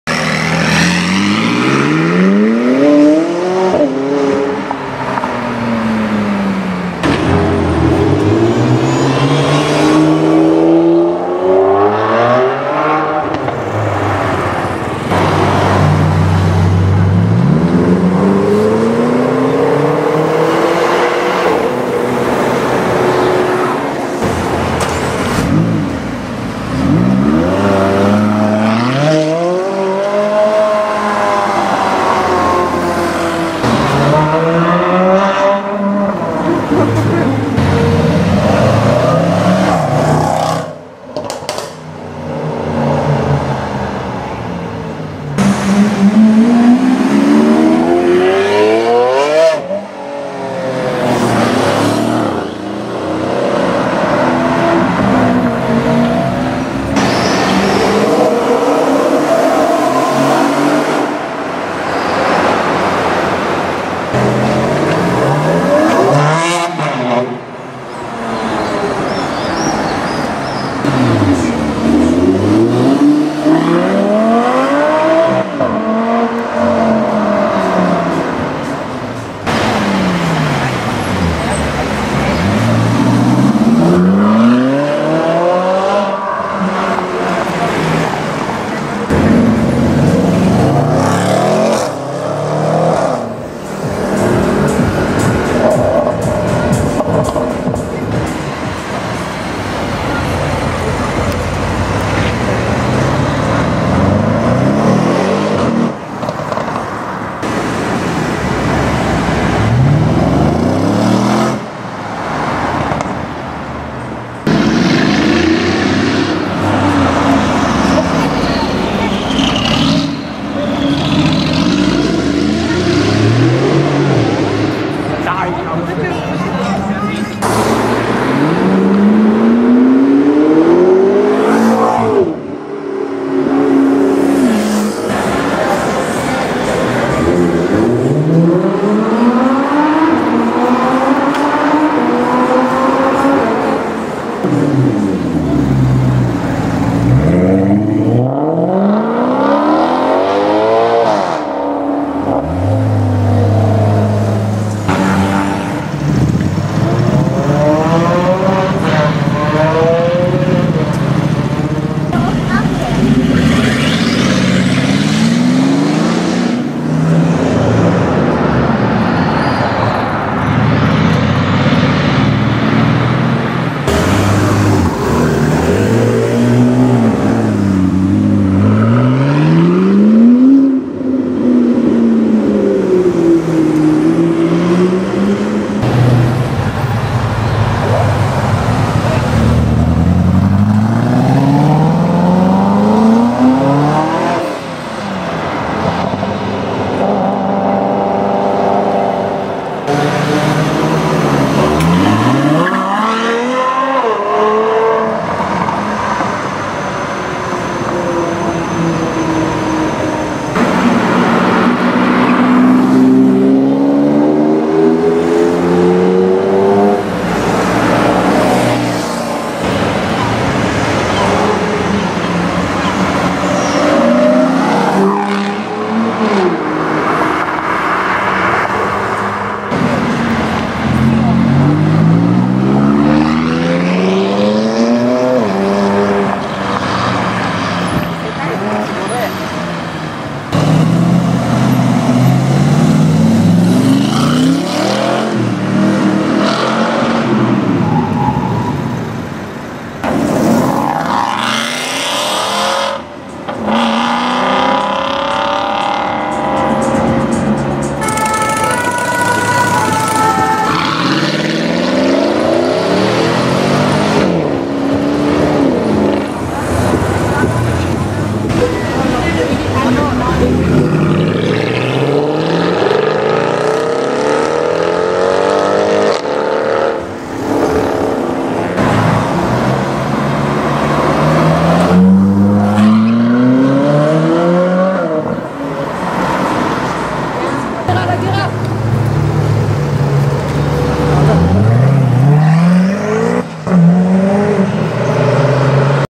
[ Mr Glenn ] Supercar Accelerating in Monaco! F40, Diablo SV, F12 tdf, Agera R, BRABUS B70..
I don't own this beat and I found this beat by the creation tool and it is created by Gunnar Olsen!

This is a Acceleration video in Monaco. All those cars in the video was in the summer 2016. You see creat cars like a FAB Design 650S, Agera R, F40, F12 tdf, 599 GTO, Huracan Spyder RS6 C7, RS7 and many more so enjoy!